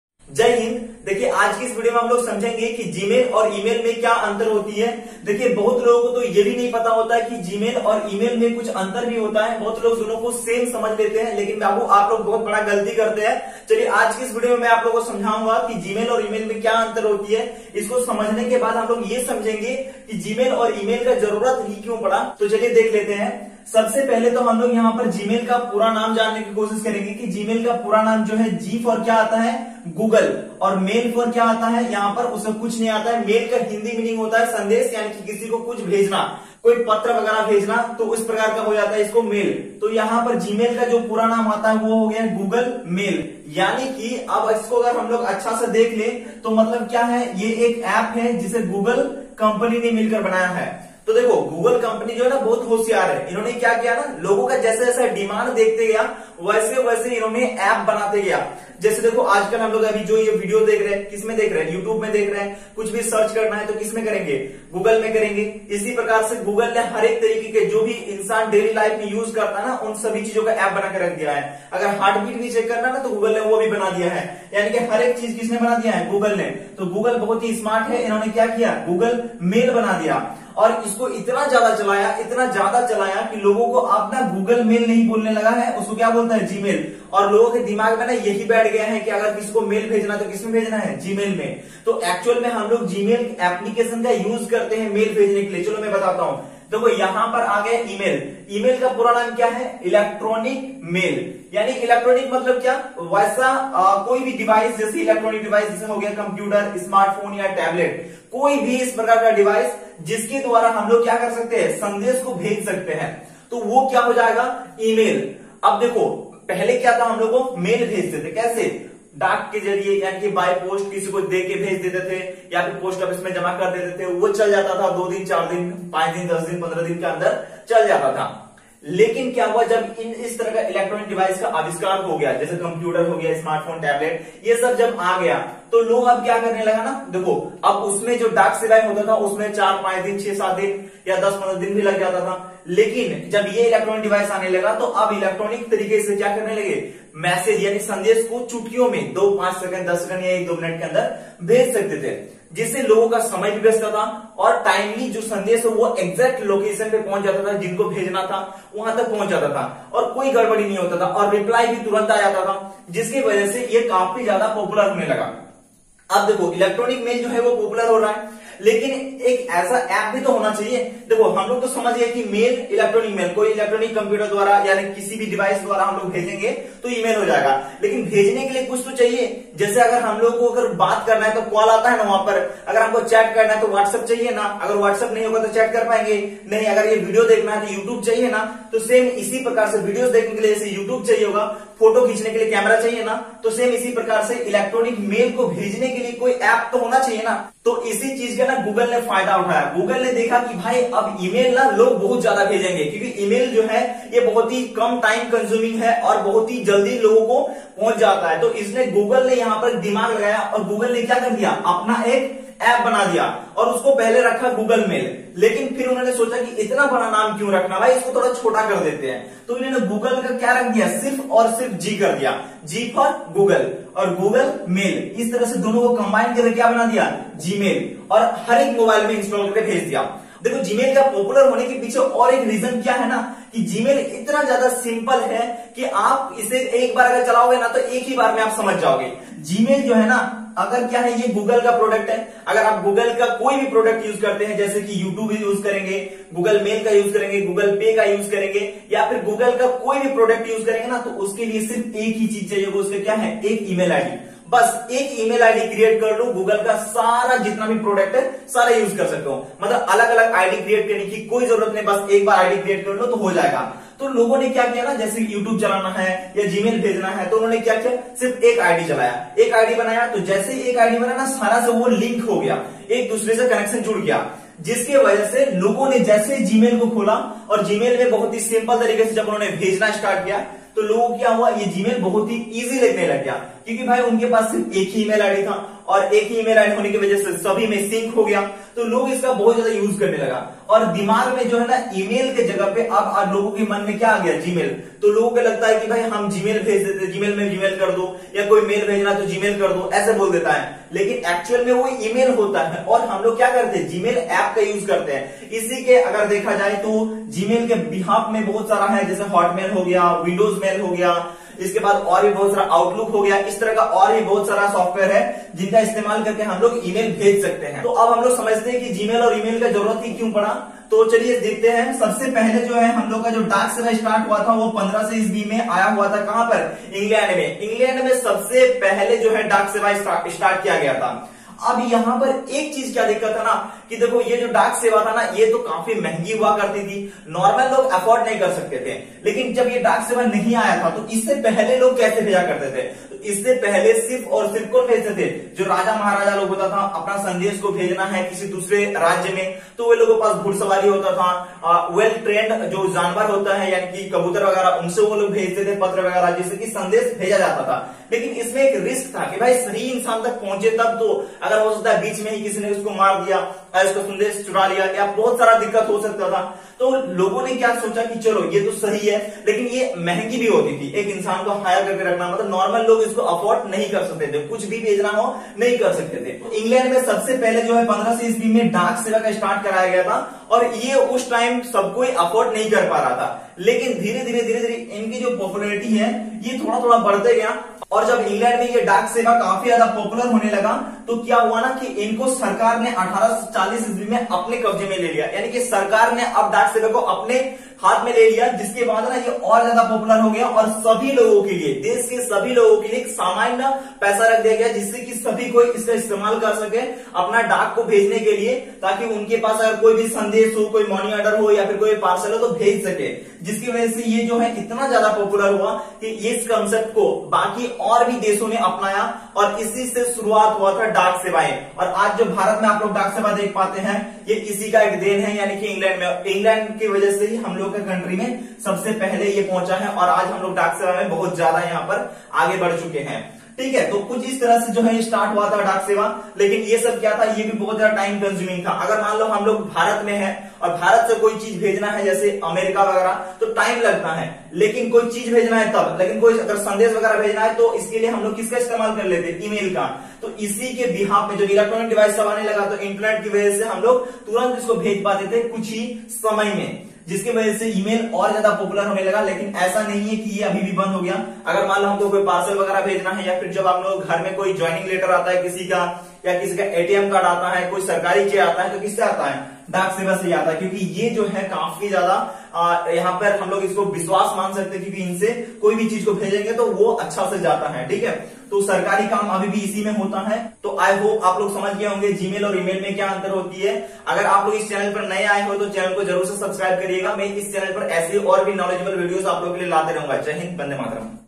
Email और Gmail में क्या अंतर है | GMail से पहले लोग कैसे Mail भेजते थे
Hello dosto is video mai maine bataya hai ki email aur gmail mai kya antar hota hai. email se pahle log kaise mail bhejte the.